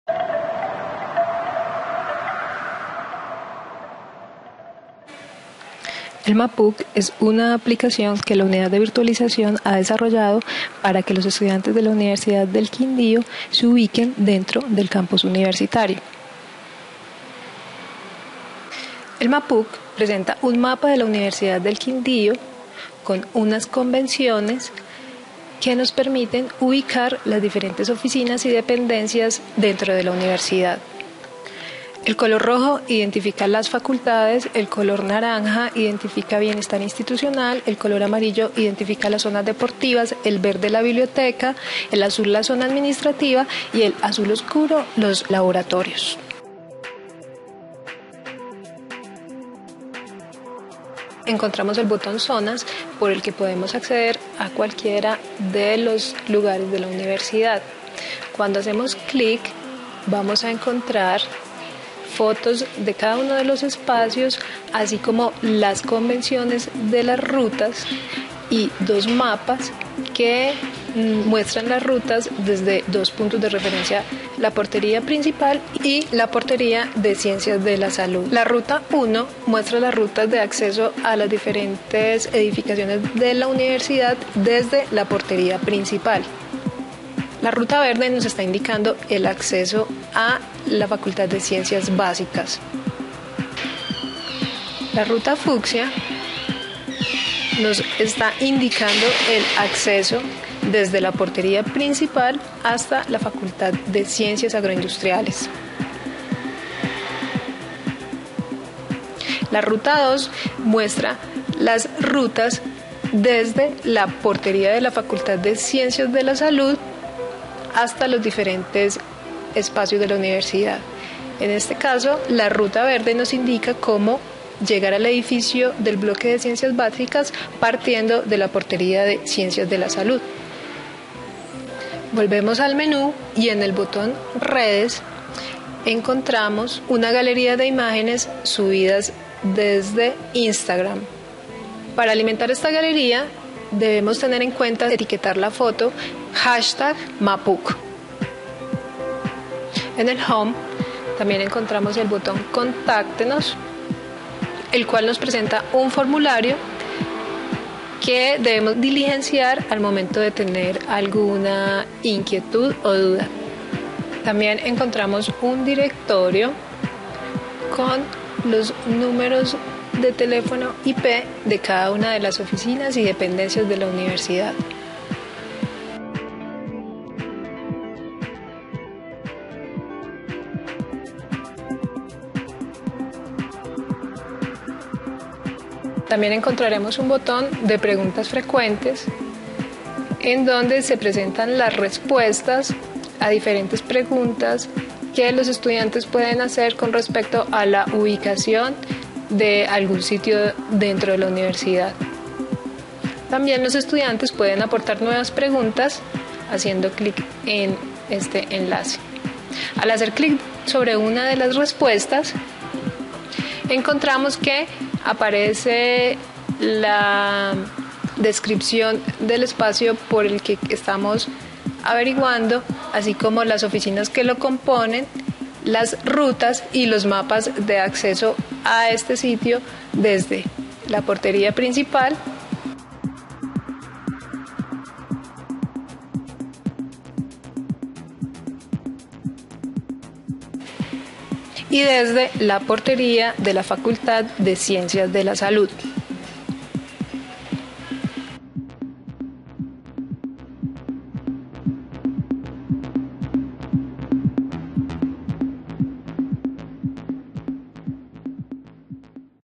¿Cómo usar el MAPUQ?
Videotutorial que explica como se usa la aplicación MAPU para que los estudiantes de primer semestre y visitantes se ubiquen en la Universidad del Quindío.